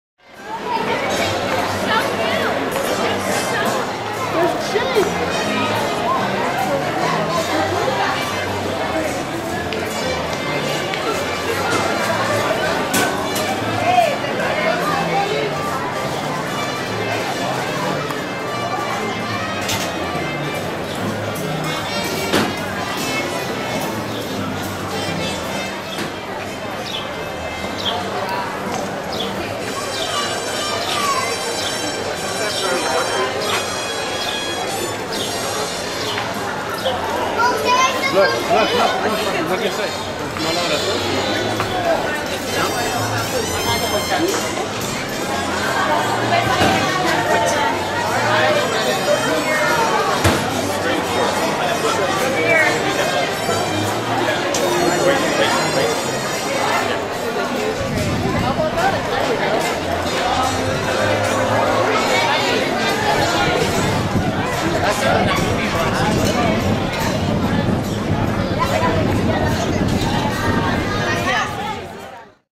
Disney California Adventures, Lone Ranger Train Invades Hollywood Land Full HD POV
As part of the premier of Disney's The Lone Ranger at Disney's California Adventures, an actual working full sized train that they used in the movie was brought to Hollywood Land to promote the film.